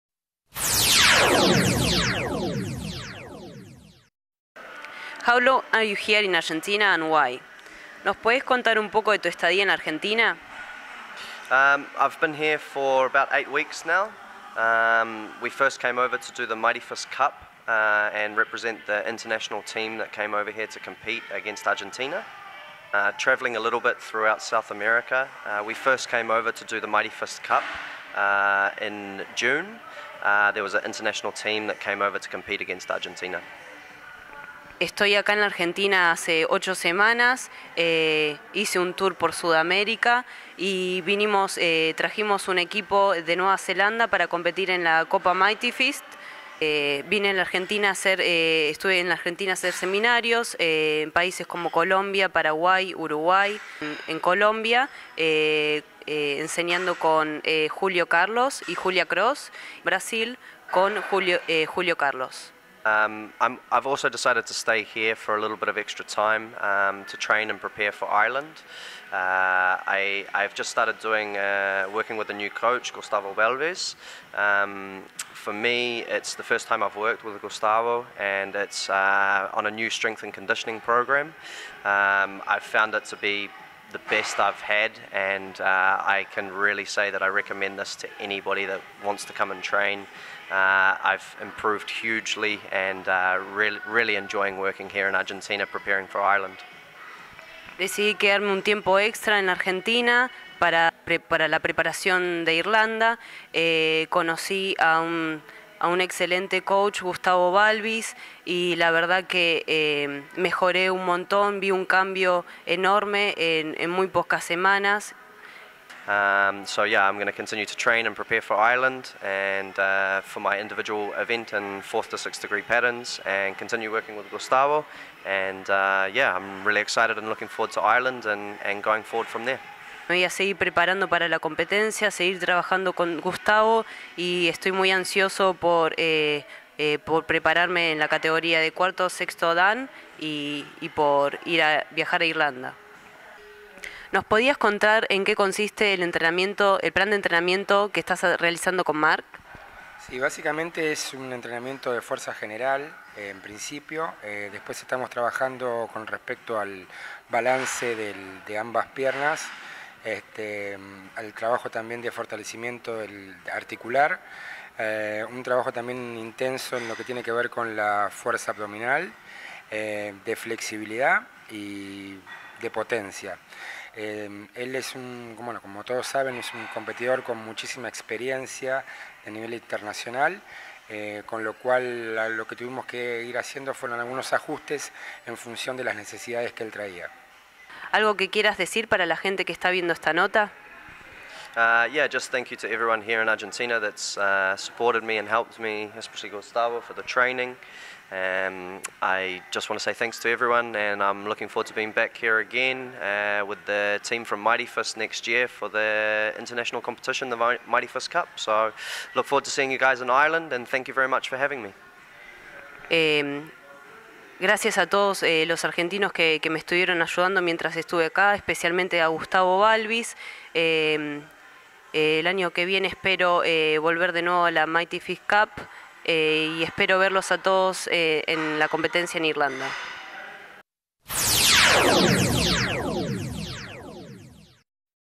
Mark Trotter preparación fisica junto a Gustavo Balbis
Durante su periodo de estadia en la Argentina el Multiple Campeón Mundial realizo un trabajo de puesta a punto fisico junto al Preparador Fisico y Profesor de Taekwon-do Gustavo Balbis